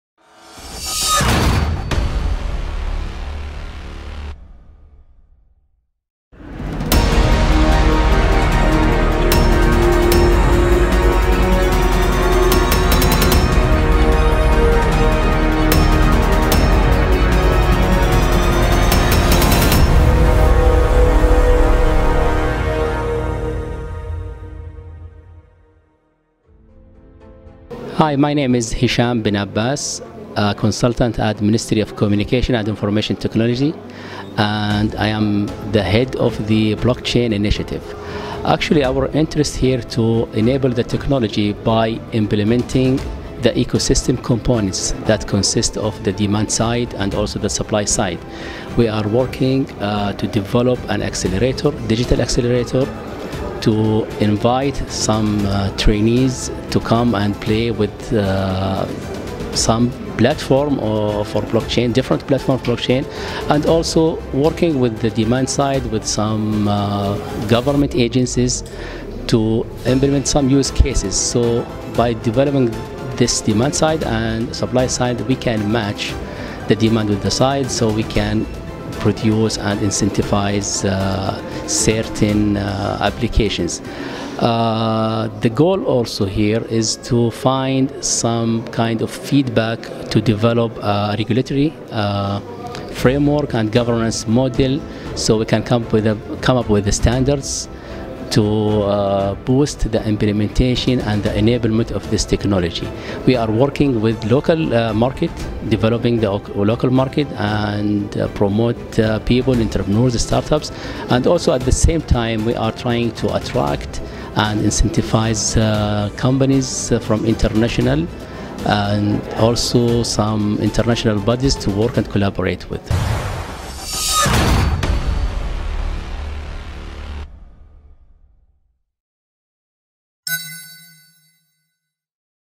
Dr Hesham Bin Abbas - Senior Consultant MCIT at UnlockBlockchain Forum Dubai
Dr Hesham Bin Abbas has helped shape companies—and in tandem—have laid the foundation for his evolution as a strong technological, project and executive director.  Devising concepts and technological solutions, presenting persuasive business cases, executing short- and long-term strategies to boost market share, and designing strategic and infrastructure services has all played a significant part of my professional make up—ensuring he deliver a rounded overview to each business decision.